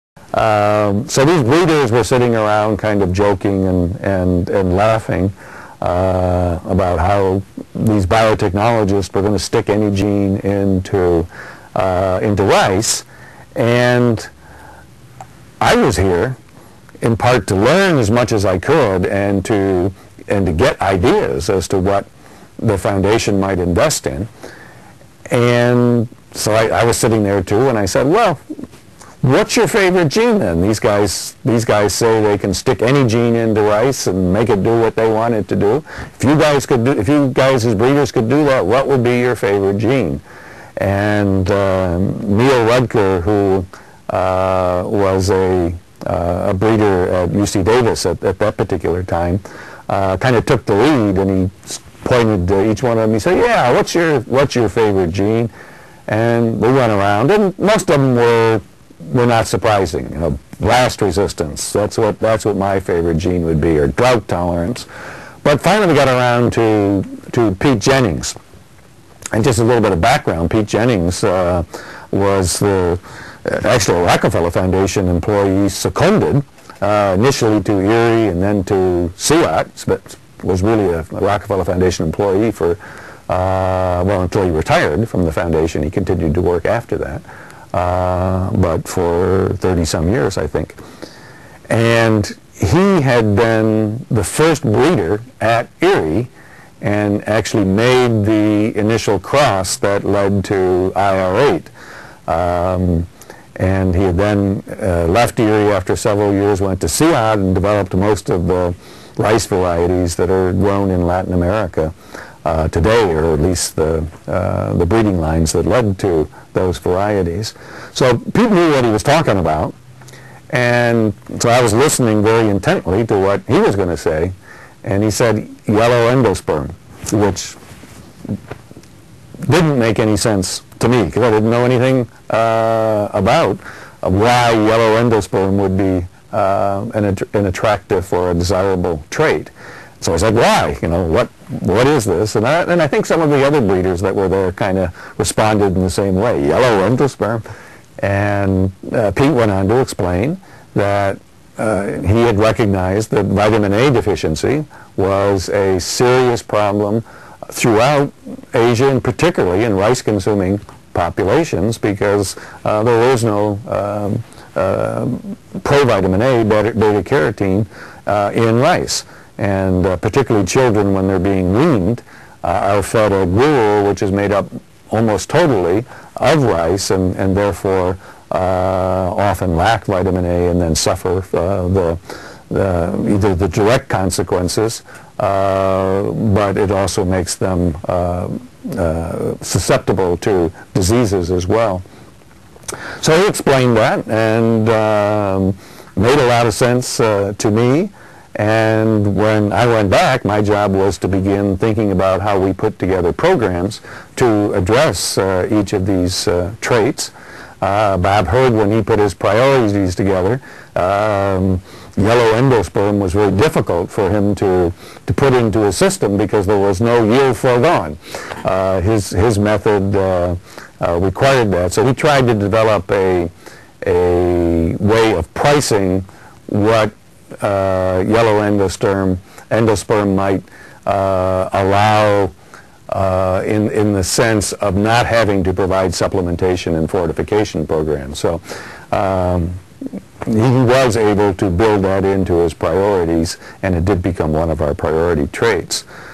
IRRI Pioneer Interviews--Yellow endosperm as a trait in rice: prelude to Golden Rice
In an IRRI Pioneer Interview Gary Toenniessen, a managing director of The Rockefeller Foundation and long-time collaborator with the International Rice Research Institute (IRRI; discusses how yellow endosperm (i.e., Golden Rice; got on the rice breeders' list of desirable traits for rice back in April 1984.

"So, these breeders were sitting around discussing how biotechnologists were going to stick any gene into rice. Now, I was at this conference at IRRI, in part, to learn as much as I could and to get ideas as to what the Rockefeller Foundation might invest in. So, I was sitting there too and I asked, Well what is your favorite gene then? These biotechnologists say they can stick any gene into rice and make it do what they want it to do. If you guys as breeders could do that what would be your favorite gene? Neil Rutger, who was a breeder at UC Davis at that time, took the lead and he asked each one in the group, Yes, whats your favorite gene?

We went around and most of them were not surprising: Blast resistance, said  one; Drought tolerance, said another. Finally, we got around to Pete Jennings. First, just a little bit of background on Pete Jennings. He was a Rockefeller Foundation employee seconded initially to IRRI and then to CIAT but was really a Rockefeller Foundation employee for 30 some years, I think. He had been the first breeder at IRRI and made the initial cross that lead to IR8. He then left IRRI after several years and went to CIAT in Colombia to develop most of rice varieties that are grown in Latin America today—or at least the breeding lines that led to those varieties.

So, Pete knew what he was talking about and, while I was listening very intently to what he was going to say, when he said yellow endosperm, it didnt make any sense to me because I didnt know anything about why yellow endosperm would be  an attractive or desirable trait. I asked, why? And I think some of the other breeders there responded in the same way. Yellow endosperm? Pete went on to explain that he recognized that Vitamin A deficiency was a serious problem throughout Asia and particularly in rice-consuming populations because there is no pro-Vitamin A or beta carotene in rice. Children, when they are being weaned, are fed a gruel, which is made up almost totally of rice and therefore they often lack Vitamin A, thus suffering the direct consequences of [possible blindness] and susceptibility to diseases.

That made a lot of sense to me. When I went back to New York, my job was to begin thinking about how we might put together research progra ms to address each of these traits. Bob Herdt, when he put his priorities together, found yellow endosperm very difficult to put into his system because there was no yield forgone. His method required that, so he tried to develop a way of pricing what yellow endosperm might allow in the sense of not having to provide supplementation and fortification programs. So, he was able to build that into his priorities and it did become one of our priority traits."